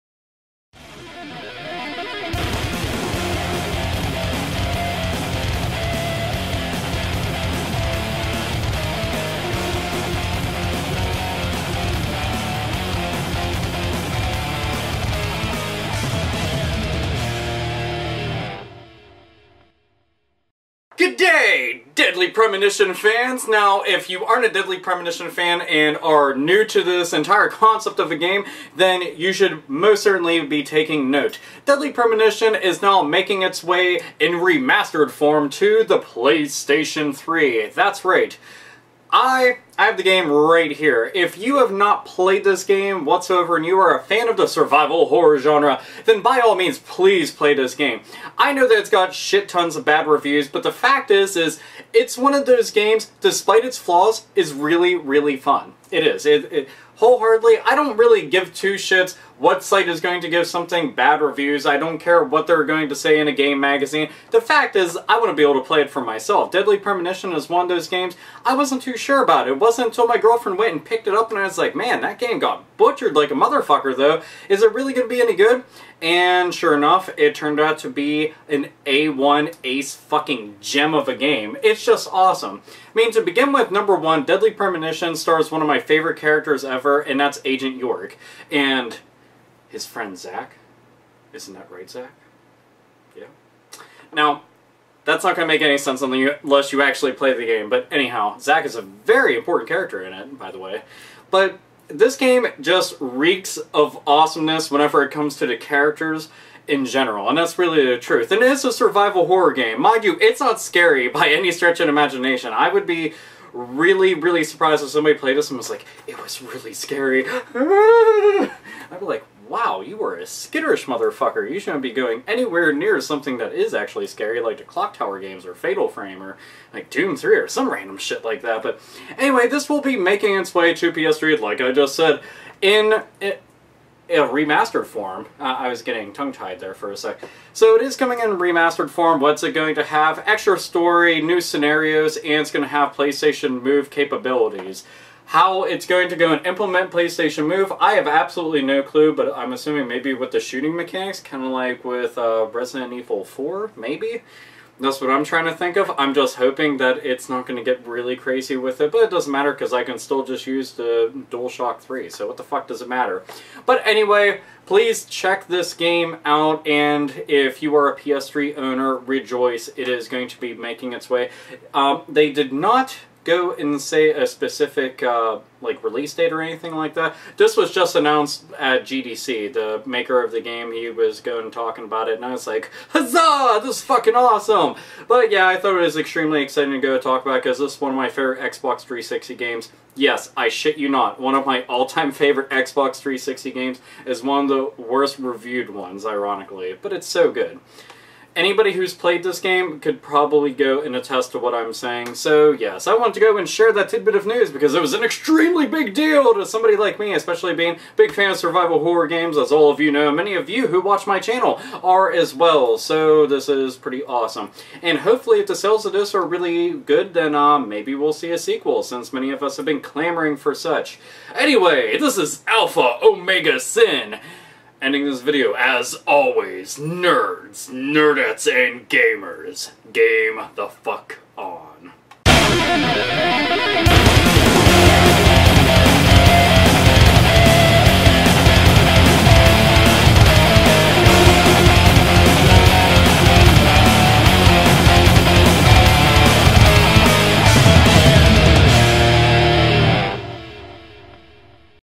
Deadly Premonition Being Remastered For PS3
One of the cult classic survival horror games will be making it's way in a remastered form to the PS3.